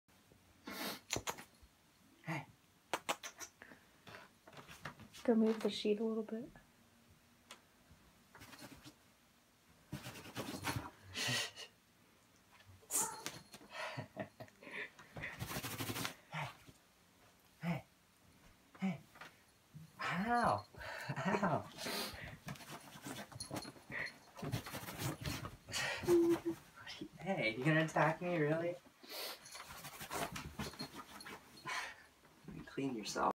Squirrel Kitty
Squirrel kitty loves fresh sheets!